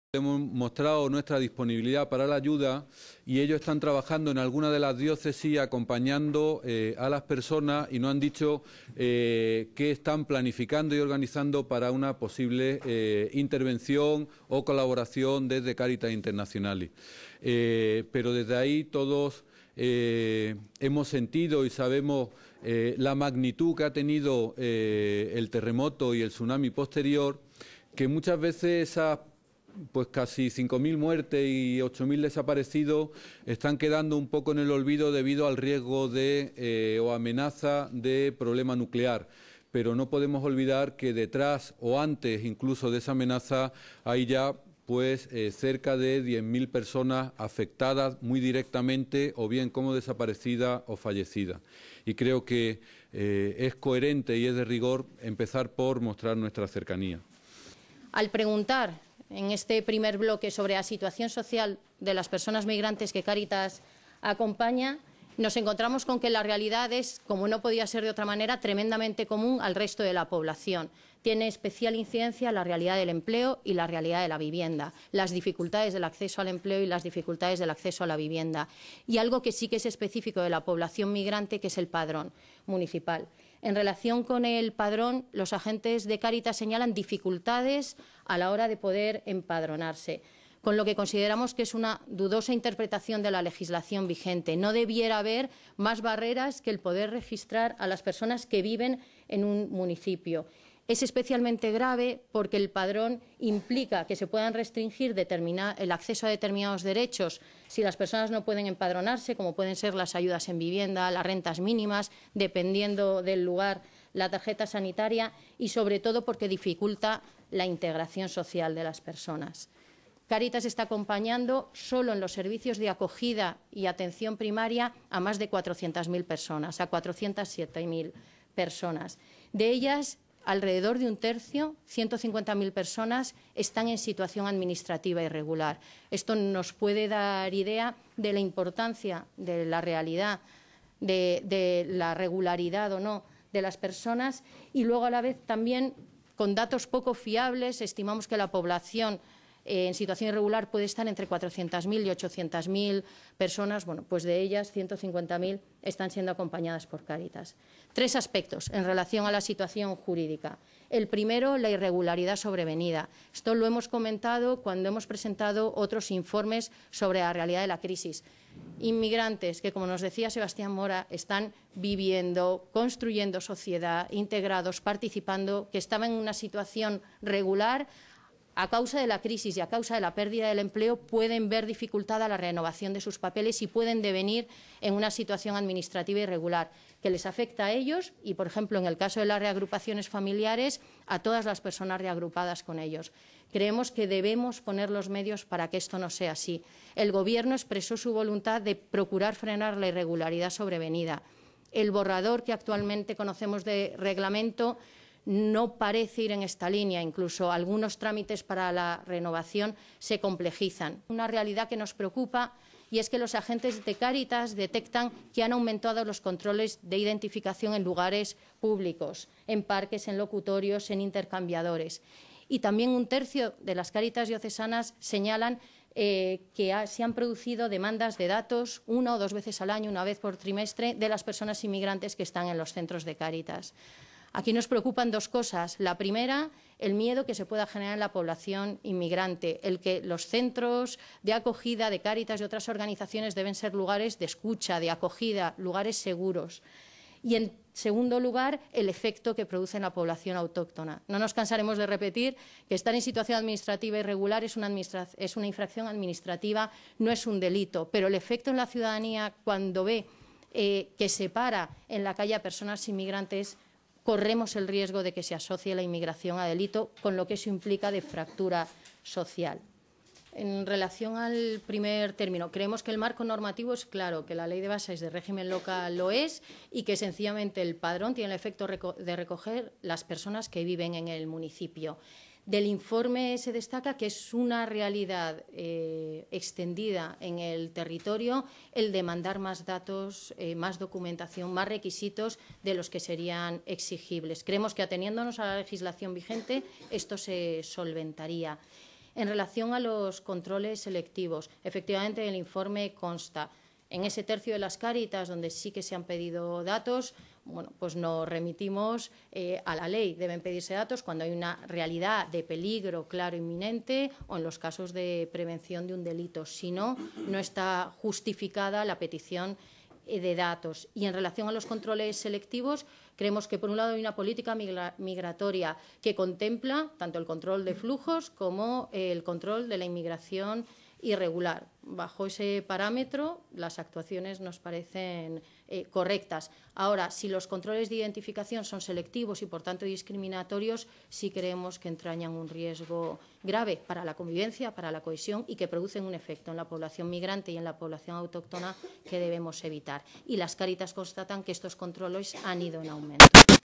Cáritas presenta su informe sobre inmigración
Declaraciones del secretario general de Cáritas España, Sebastián Acora, presentando el informe de la organización humanitaria sobre la situación social de los inmigrantes atendidos por la institución.